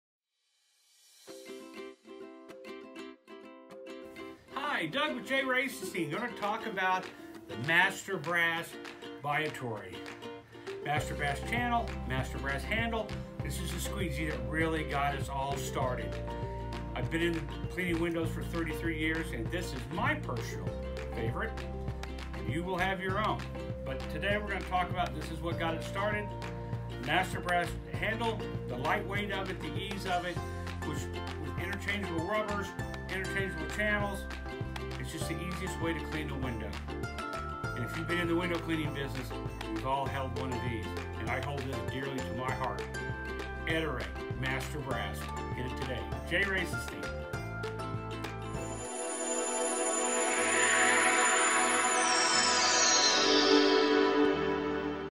Handle Master Brass Ettore
Have a question? Get answers on the forum!

Ettore imagined the Bulb Channels and culminated the right blend of weight versus strength.

Each Channel is hand assessed guaranteeing indisputably the best and faultless exhibition.

Ettore channels is the ideal harmony between weight and flexibility, all Ettore Channels can be joined with different Ettore wiper handles.

Each Channel has the Elastic introduced.
Channels fits any Ettore wiper handles
Ace Metal has the ideal equilibrium and weight
Tempered Steel offers a lighter vibe with eminent inflexibility.
Aluminum offers a featherweight coast to your work
Made of strong tempered metal